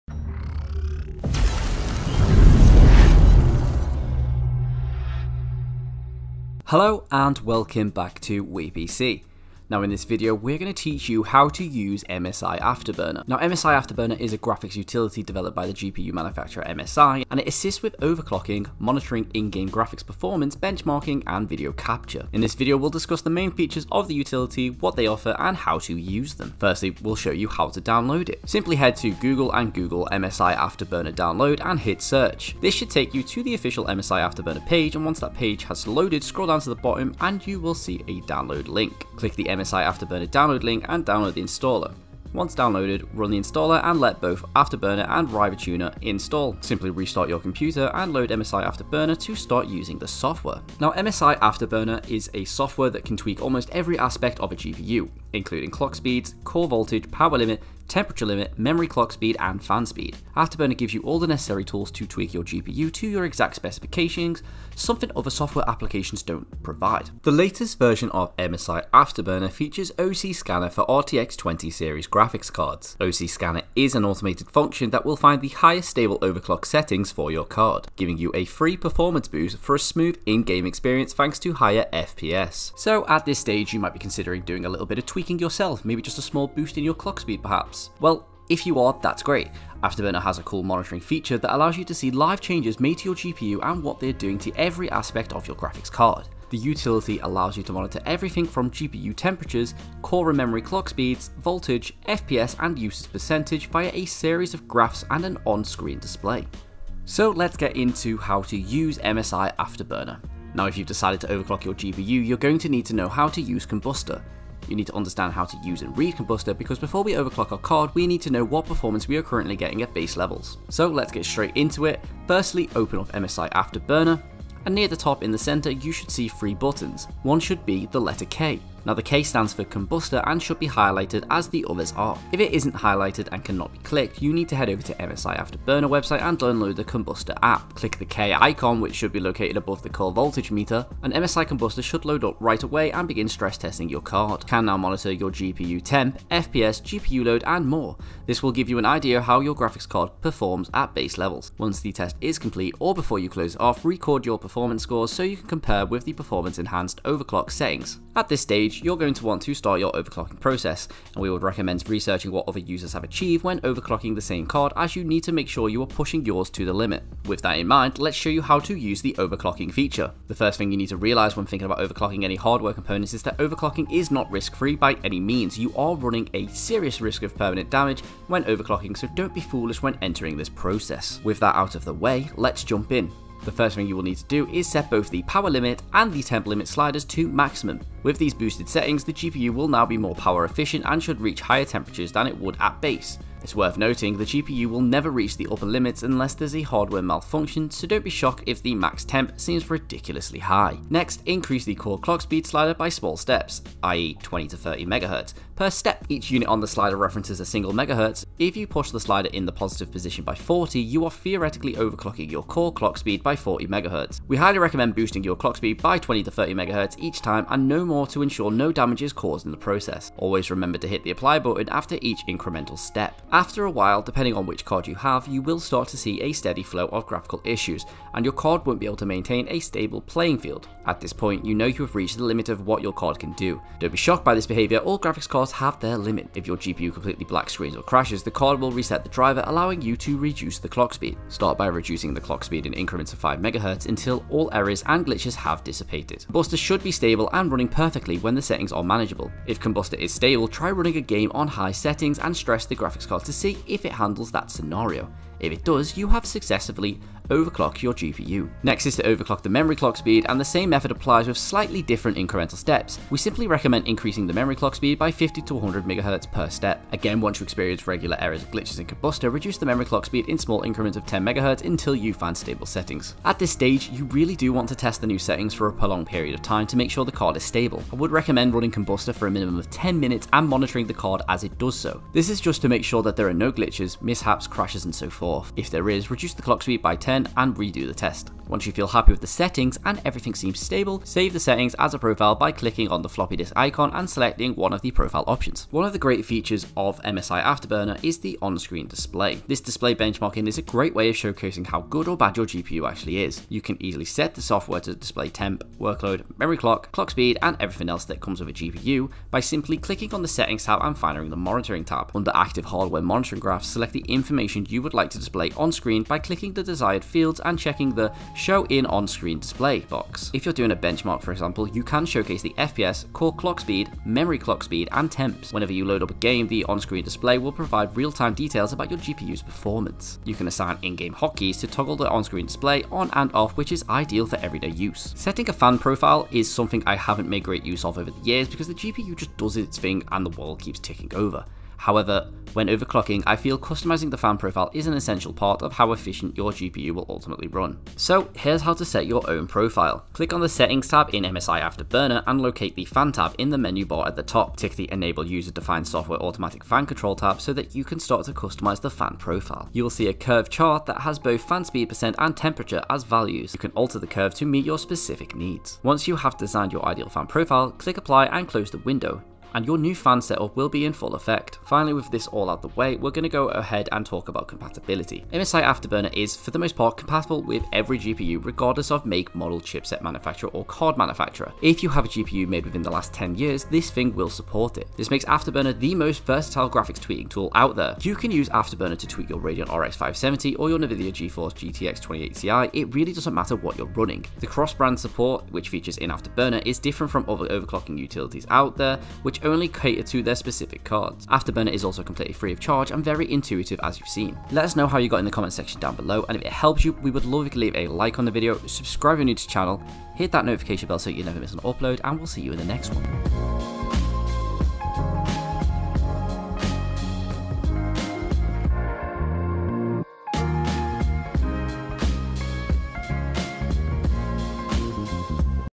How To Use MSI Afterburner | Overclocking, Custom Fan Curves and Stability Testing | WePC
Don't know how to use MSI Afterburner? Don't worry, we'll show you how in this handy video.

In our how to use MSI Afterburner video tutorial we walk you through configuring MSI Afterburner and using it to overclock your GPU as well as create a custom fan curve. We briefly look and Nvidia's OC Scanner which allows for a one-click overclock as well as how to overclock with MSI Afterburner yourself. When manually overclocking you want to bump the core clock in 25MHz increments and the memory clock in 50MHz increments until the system is no longer stable - graphics card artifacts or display driver crashing. If you encounter artifacts or crashing then back off on the clocks to the previous stable frequency and perform a 10 minute stress test with MSI Kombustor to ensure stability. Once you have found the overclock to be stable in MSI Kombustor, check further with a few games.

👉For the latest PC Gaming Hardware reviews and gaming news, find us